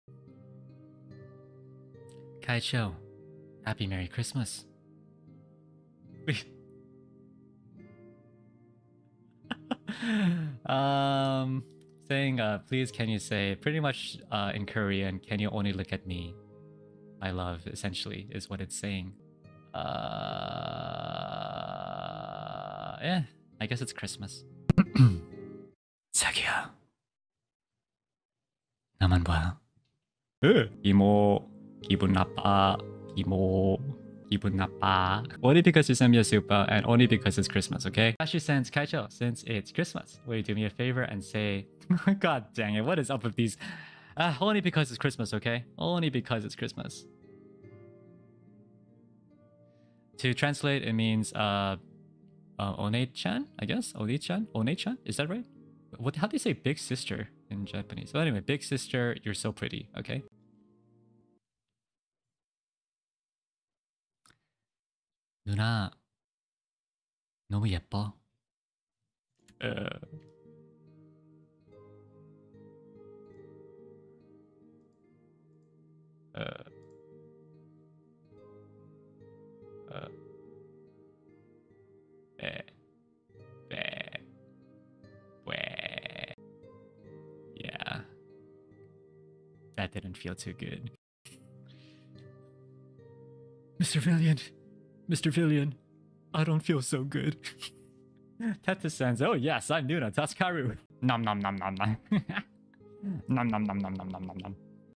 【彩虹社EN】會長聖誕韓文大放送滿足粉絲願望 溫柔的韓國歐爸下秒立刻直男癌發作！？ 【Ver Vermillion】【Vtuber精華】
有任何對影片的建議請不吝指教～